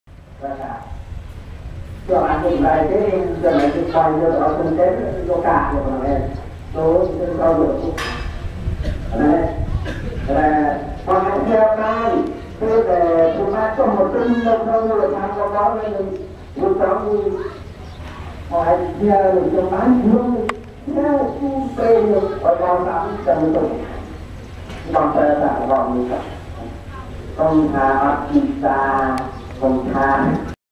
Kambodschanische Sau Cambodian Pig
mit ihrem Nachwuchs. Gefilmt in einem Wat unter der Versammlungshalle.